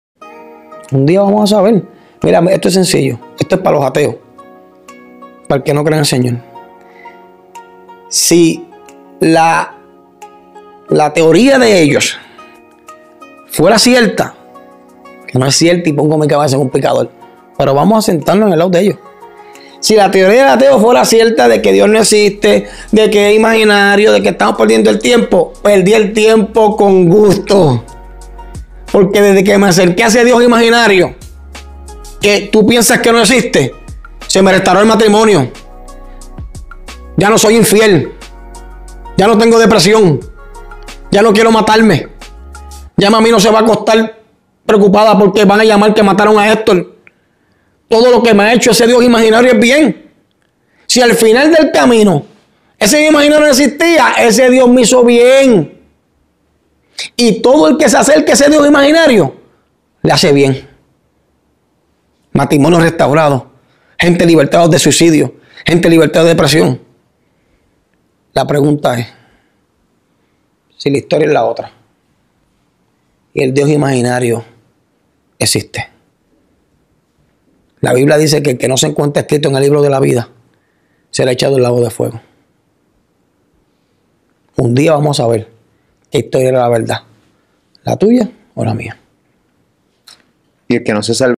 excantante Hector El Father, MANDA UN MENSAJE PARA LOS ATEOS
creditos al video a moluscotv en un  podcast lo invitan a excantante Hector El Father y manda un me saje a loas ATEOS